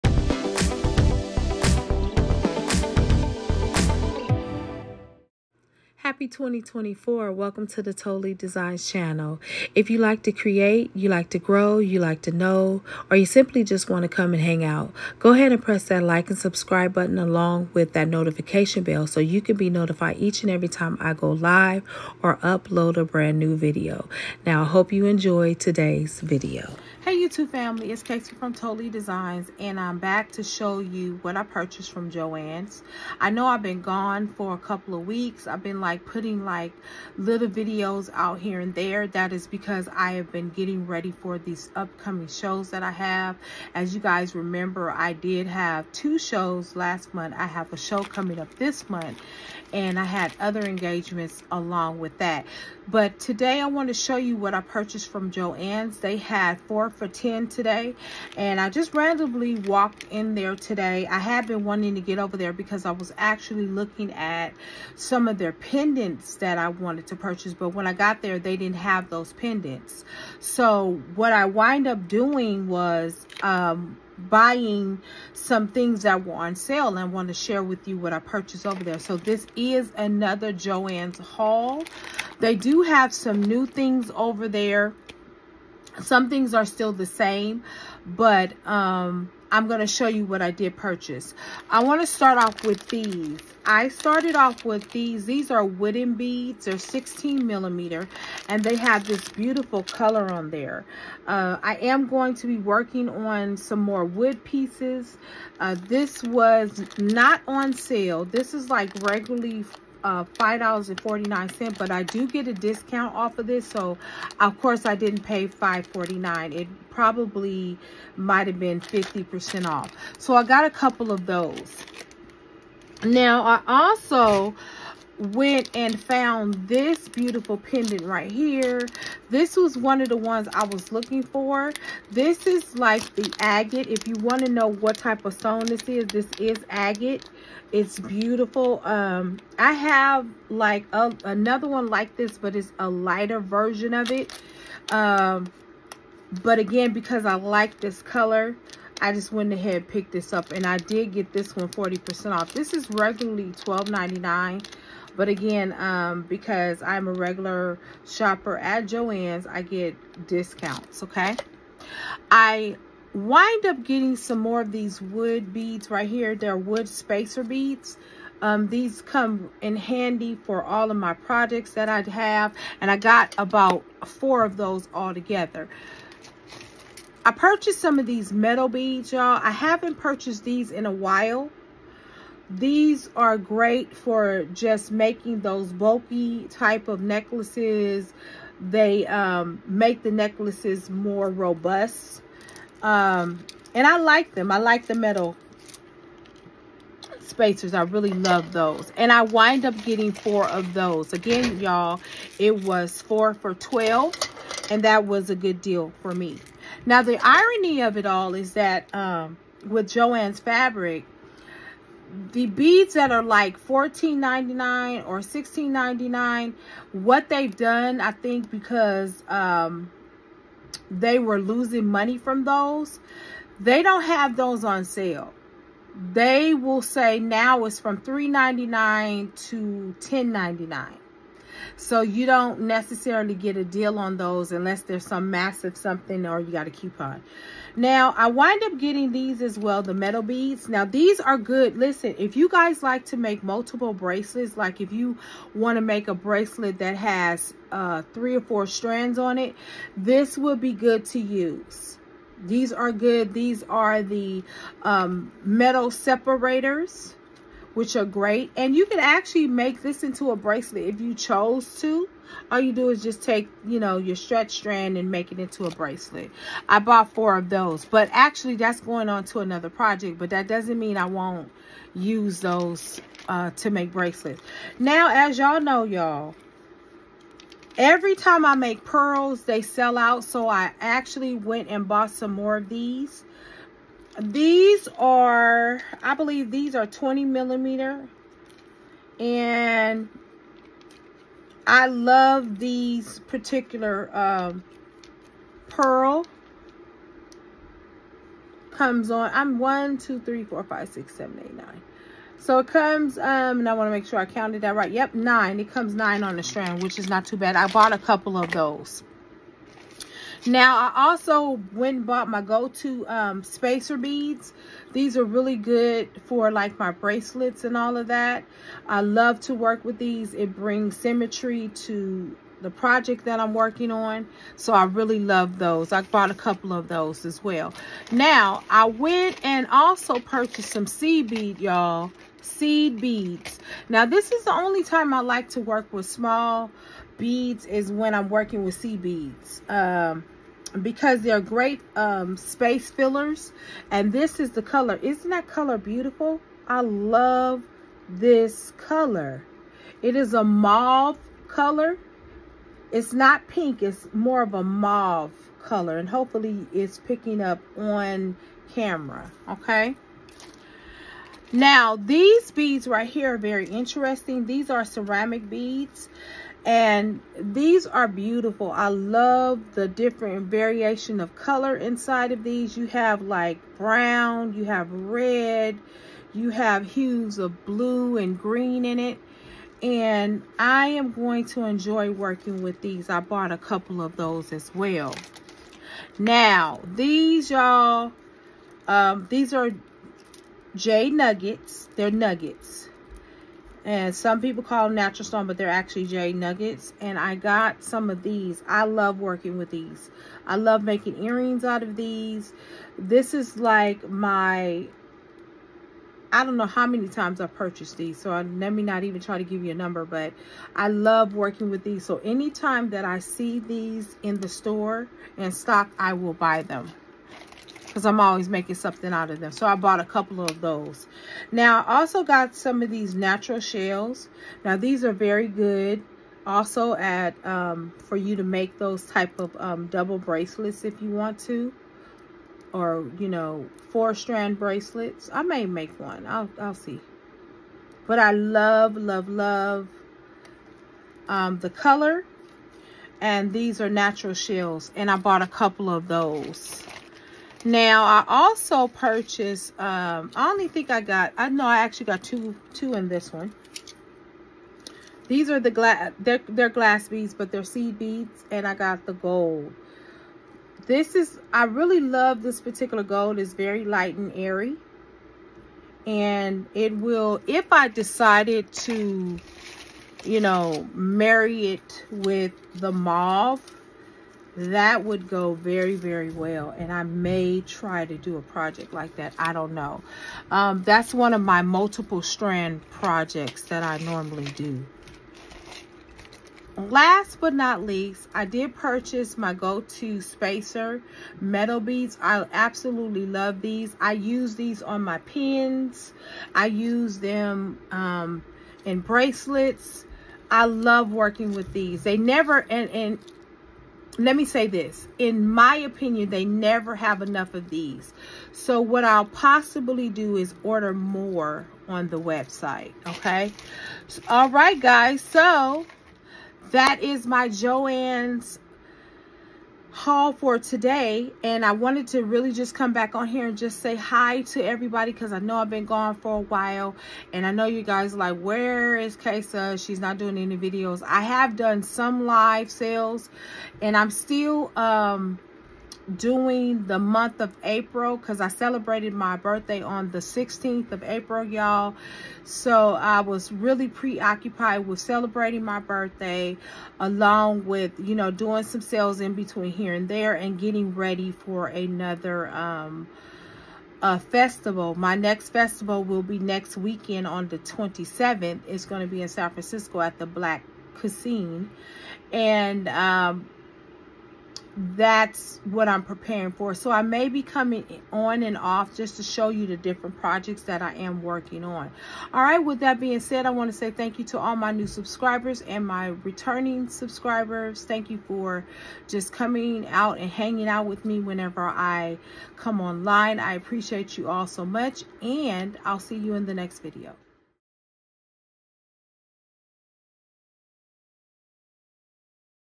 Joann 4 for $10 Sale
Today’s video will feature the latest sale and the newest beads from Joann.

To purchase my book on vending